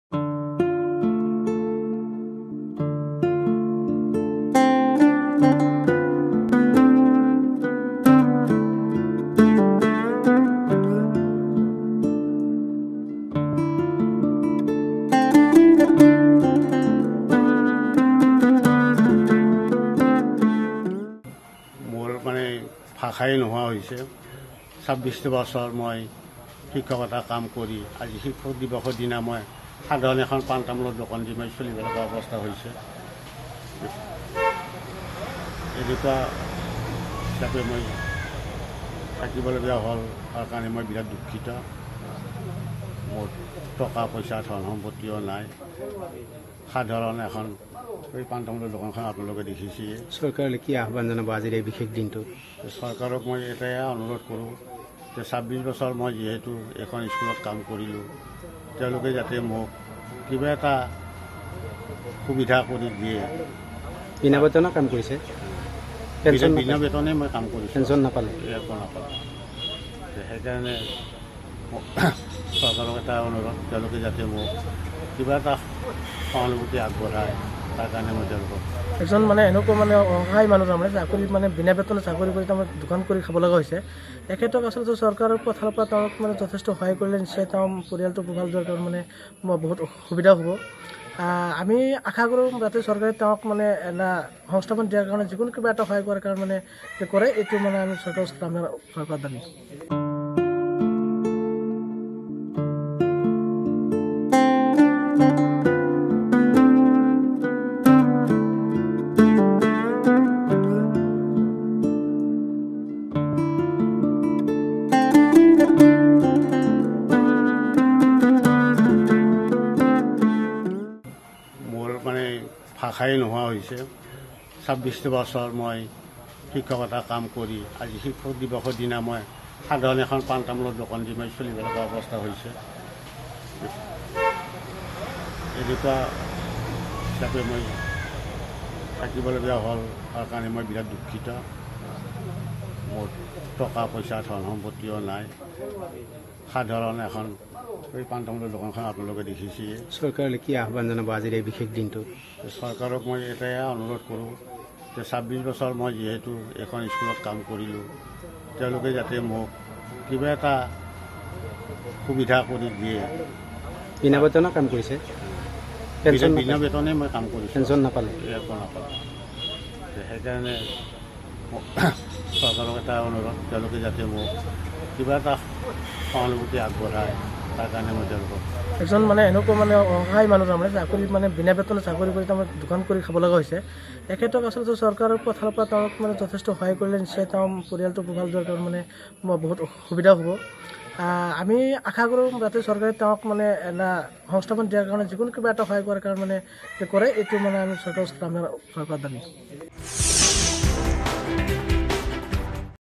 শিক্ষক দিৱসৰ বিশেষ দিনটোতে সেমেকি উঠিল এগৰাকী সদ্য অৱসৰপ্ৰাপ্ত শিক্ষকৰ দুচকু
শিক্ষক দিৱসৰ বিশেষ দিনটোতে সেমেকি উঠিল এগৰাকী সদ্য অৱসৰপ্ৰাপ্ত শিক্ষকৰ দুচকু। বিনা বেতনে ২৬ বছৰে শিক্ষকতা কৰি নমস্য শিক্ষাগুৰুৱে ভাটিবয়সত পৰিয়াল চলাইছে পদপথত পান দোকান দি